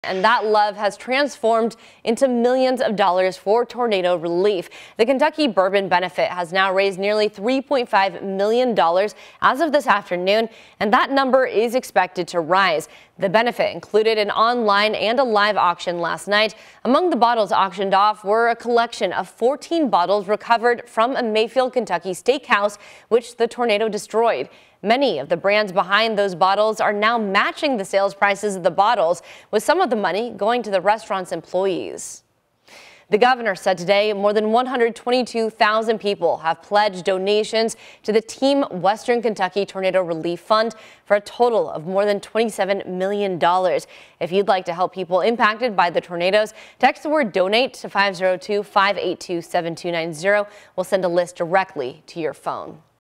Kentucky Bourbon Benefit raises money for tornado relief efforts
Among the bottles auctioned off were a collection of 14 bottles recovered from a Mayfield, Kentucky steakhouse which the tornado destroyed.